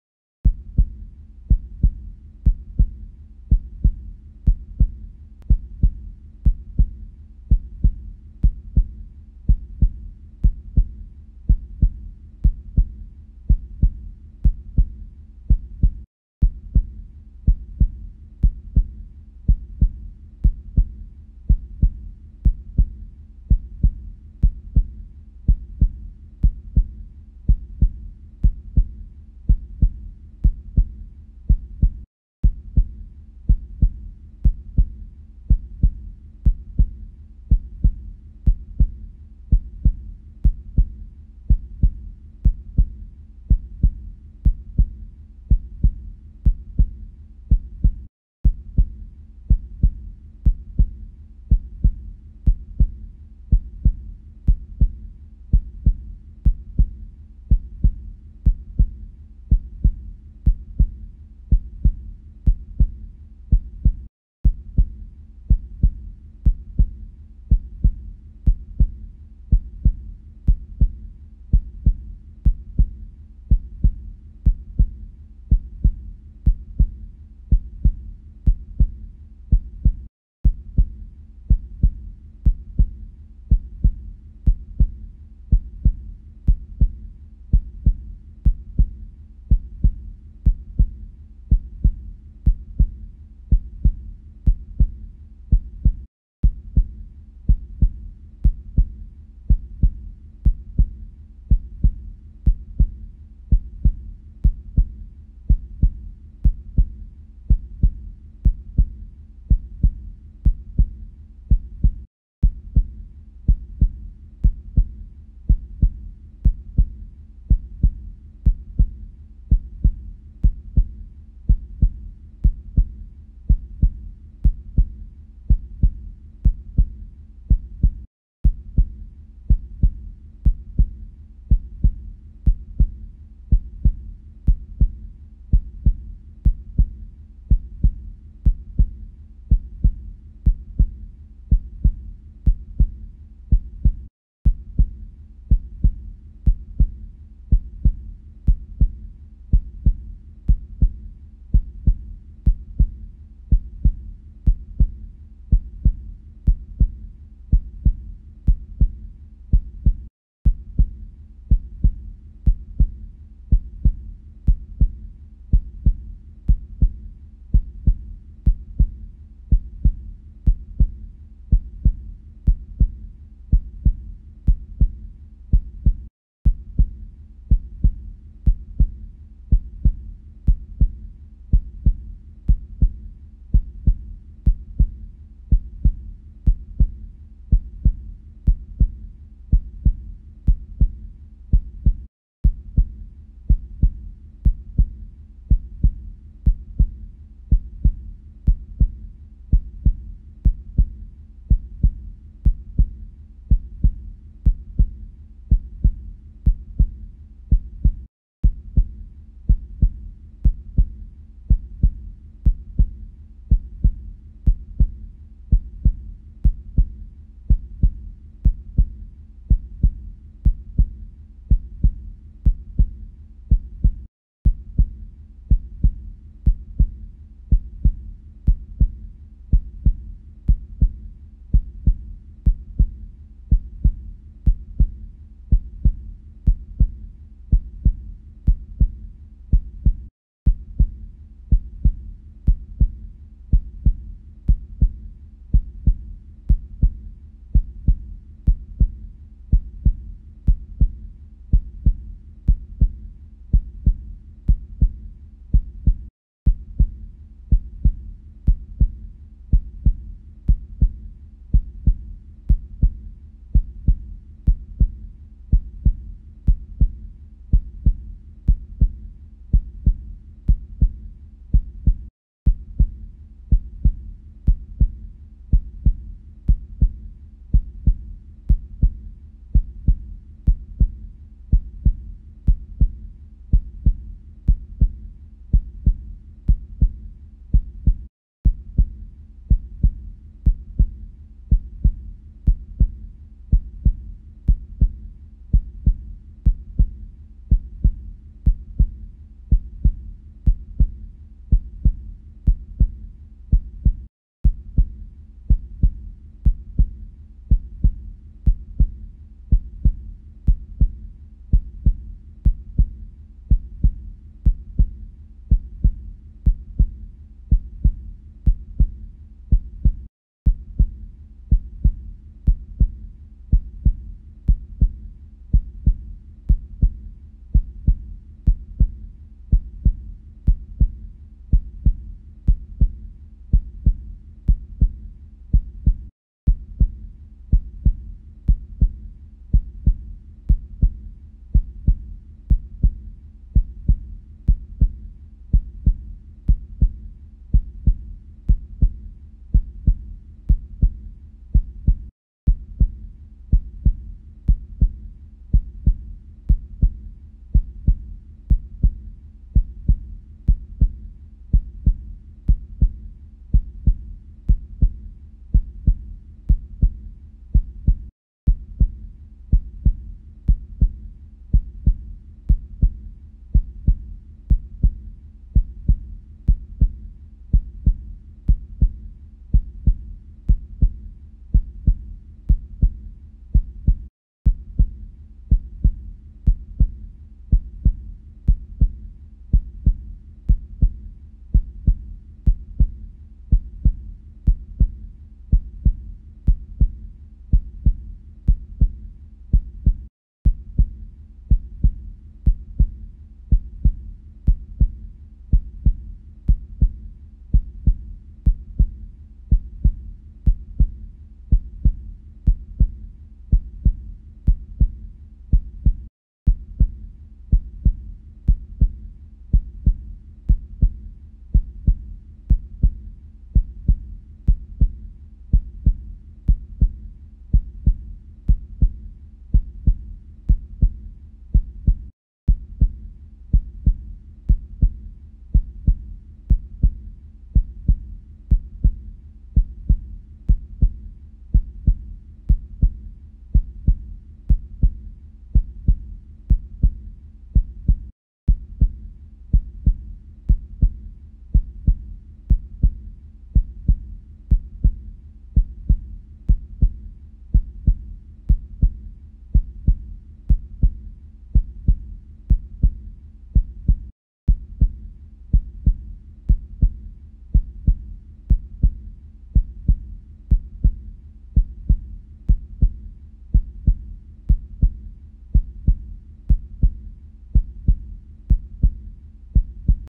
EYE vibrations. up. To the left. To the right. Heart RHYTHM!
Strengthening of OCULOMOTOR MUSCLES and displacement of the assemblage point of VISION on the retina in the rhythm of the heart and with full breathing of YOGIS!